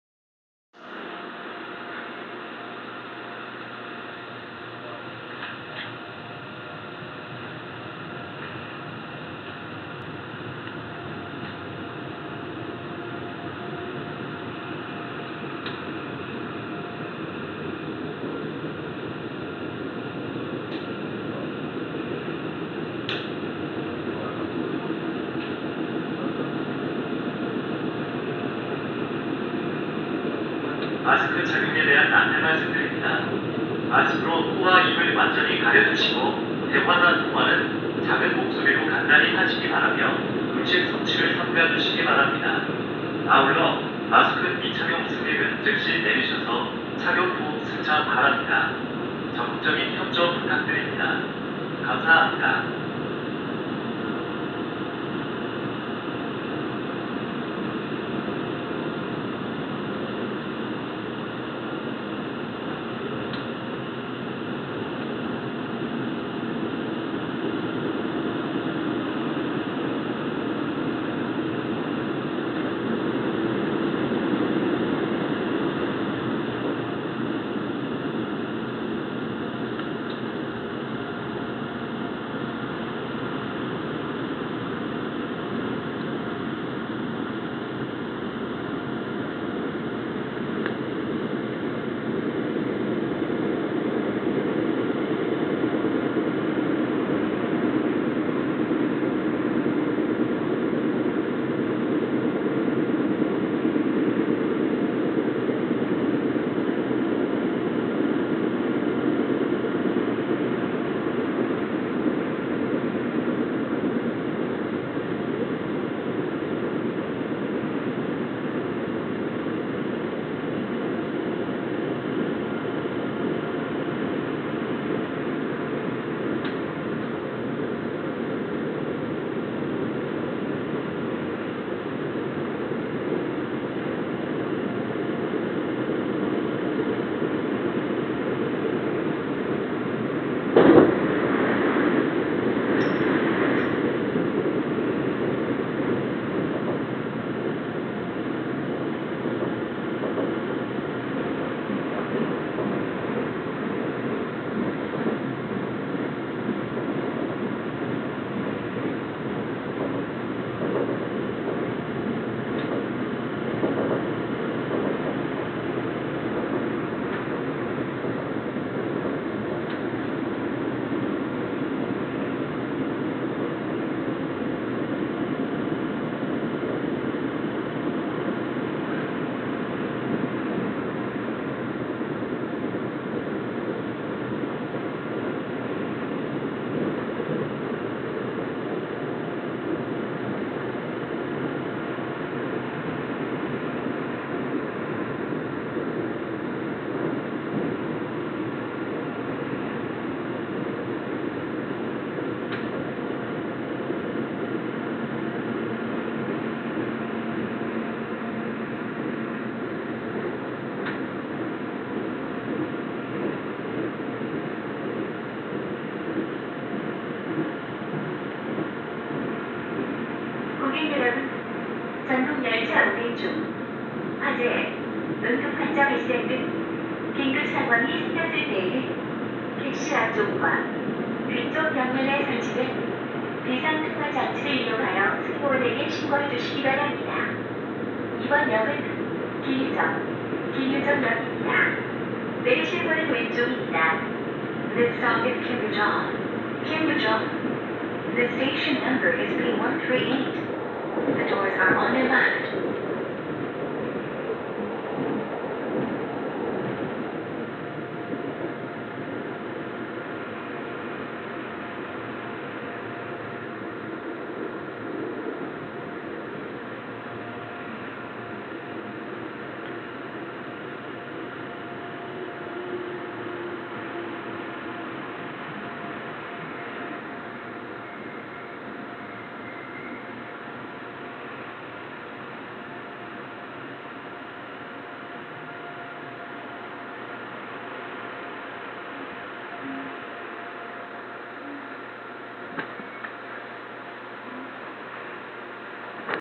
한국철도 수도권 전철 경춘선 상봉행 열차 경춘선 남춘천역~김유정역
20년 8/22 촬영